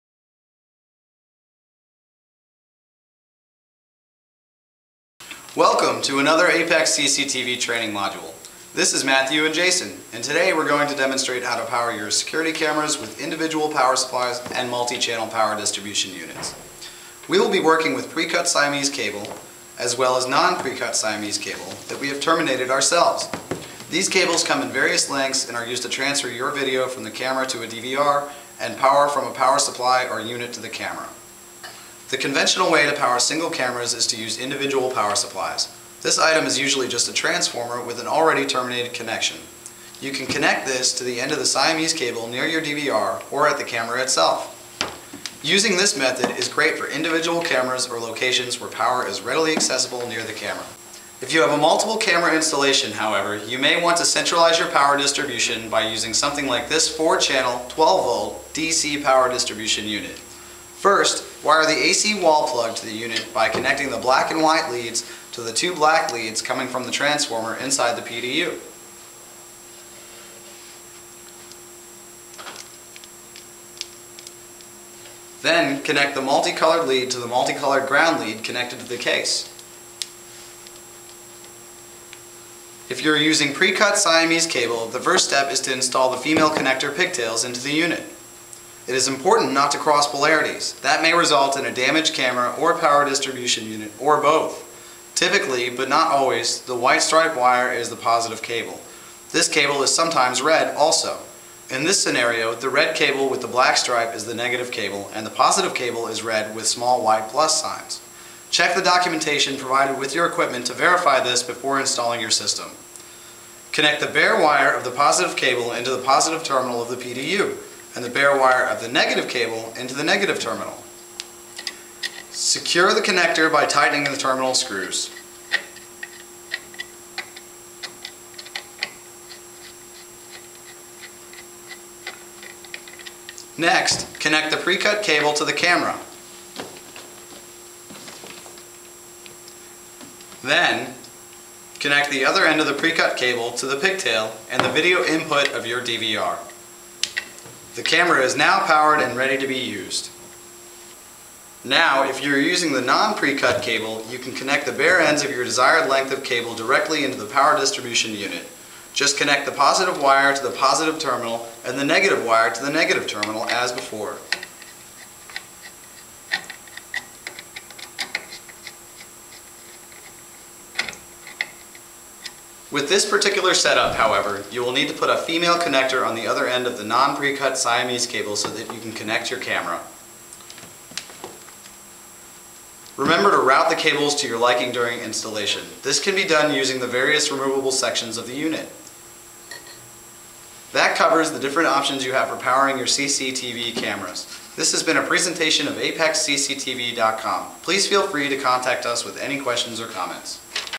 Video Tutorial : How to Power Your CCTV Security Cameras
- CCTV Video Tutorials by the Security Camera Experts

This video demonstrates the different options you have when powering your CCTV cameras and how to connect them to your CCTV power supply. Whether you have pre-cut siamese cable or RG59 on a spool leading to a 12v DC security camera power supply or 24v AC CCTV power supply, the fundamental connections remain the same as demonstrated in the video.

There are many ways to power your security cameras, but please make sure that you are using the correct voltage. If you wire an AC power supply to a DC camera, that CCTV camera will be overloaded and short-circuit.

Please let us know if the video was helpful by leaving comments in the area below and giving us a thumbs up!